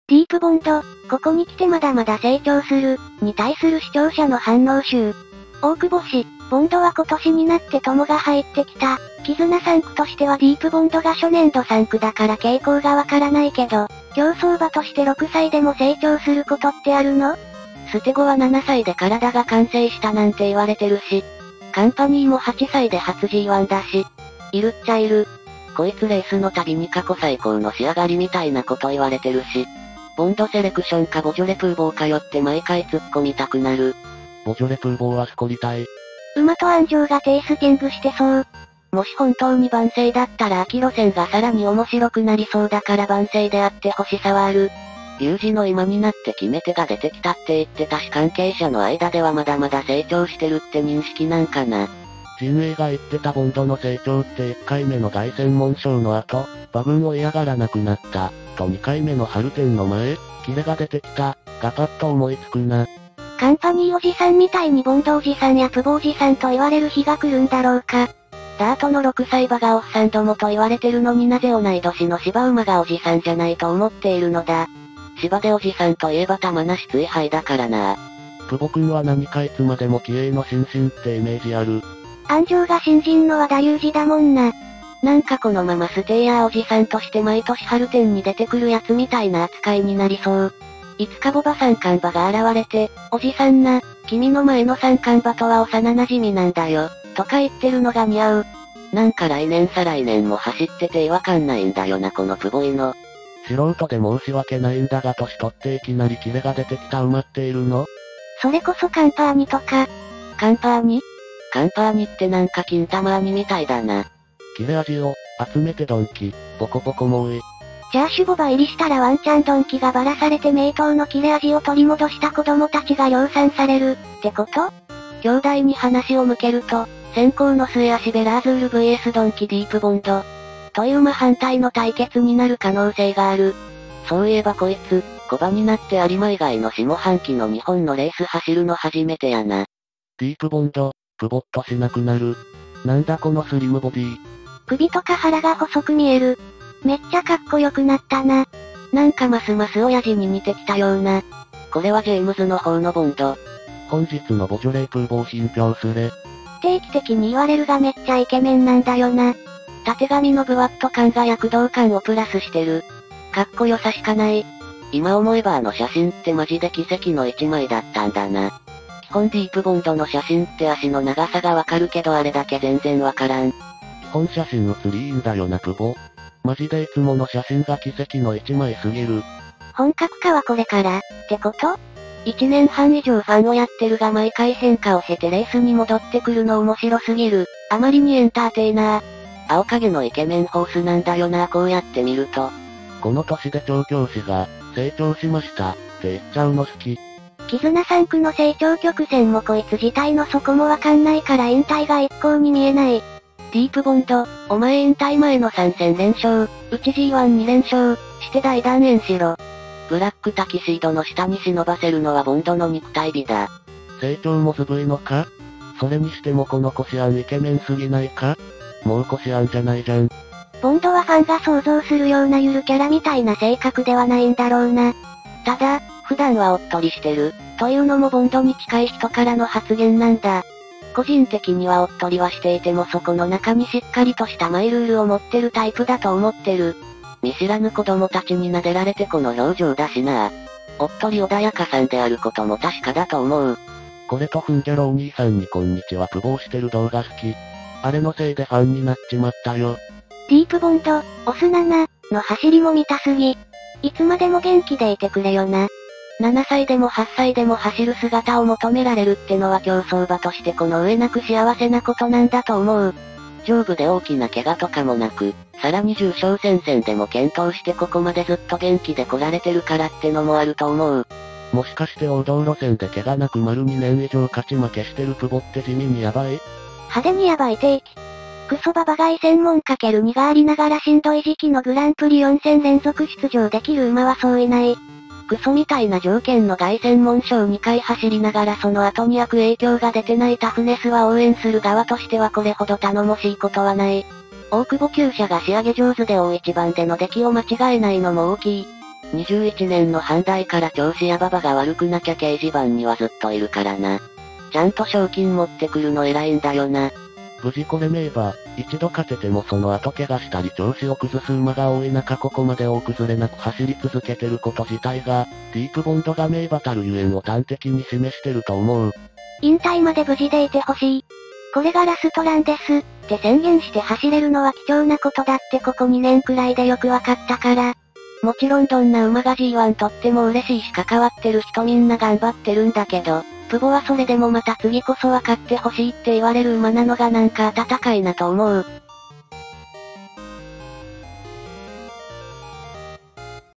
【競馬の反応集】「ディープボンド、ここに来てまだまだ成長する」に対する視聴者の反応集
今回は「ディープボンド、ここに来てまだまだ成長する」に対する視聴者の反応集について紹介しました。

引用

大久保師「ボンドは今年になってトモが入ってきた」

を使用しています。そのほかの楽曲は一切使用していません。